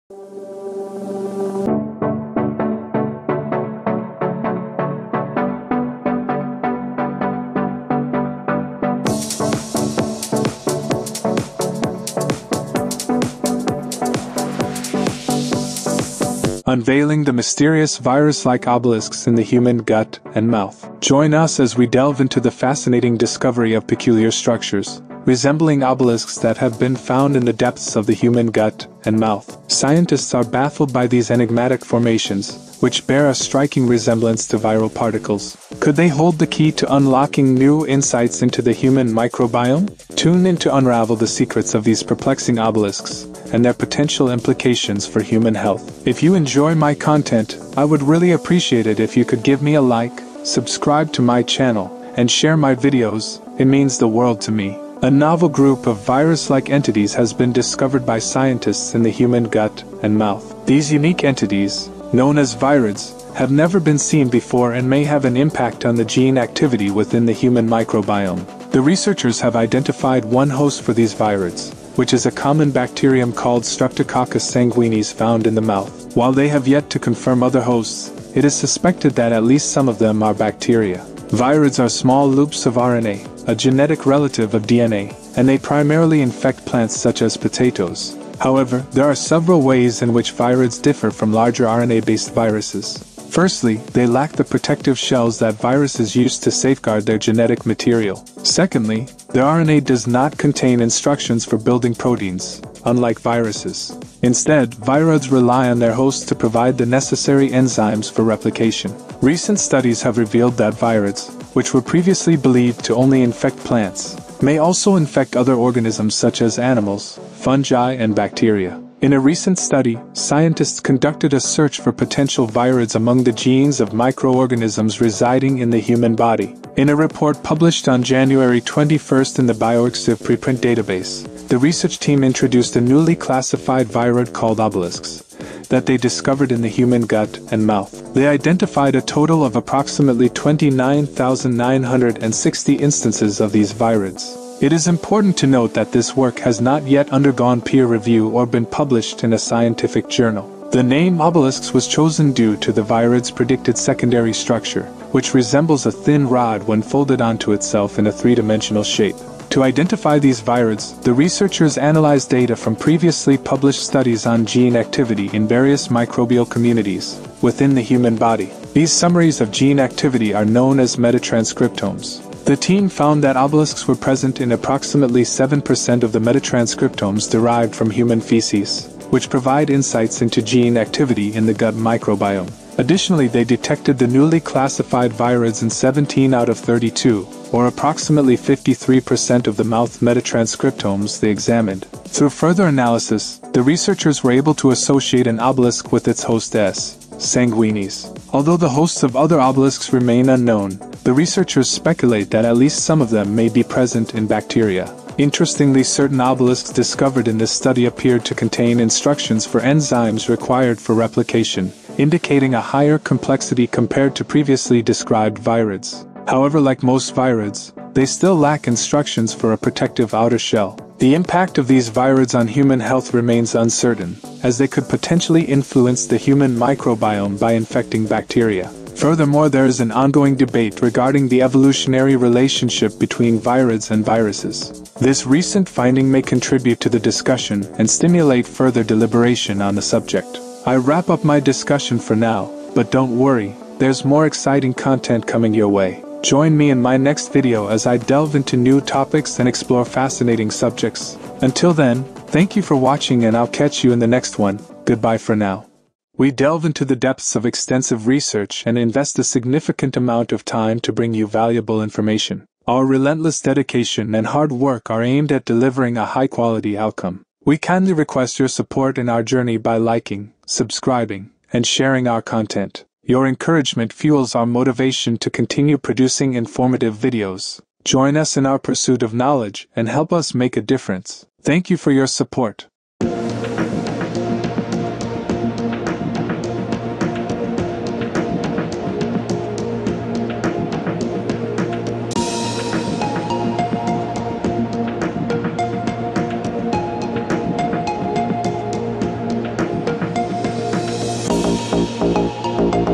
Are 'Obelisks' in the Gut and Mouth Contagious? How Can They Be Transmitted ?
Join us on our YouTube channel as we dive into the fascinating world of the human gut and mouth. In this series, we explore the intriguing discovery of virus-like obelisks that have left scientists perplexed.
These peculiar structures bear a striking resemblance to viral particles, sparking curiosity about their role in the human microbiome.
Don't miss out on unravelling the secrets of these enigmatic obelisks and their potential implications for human health.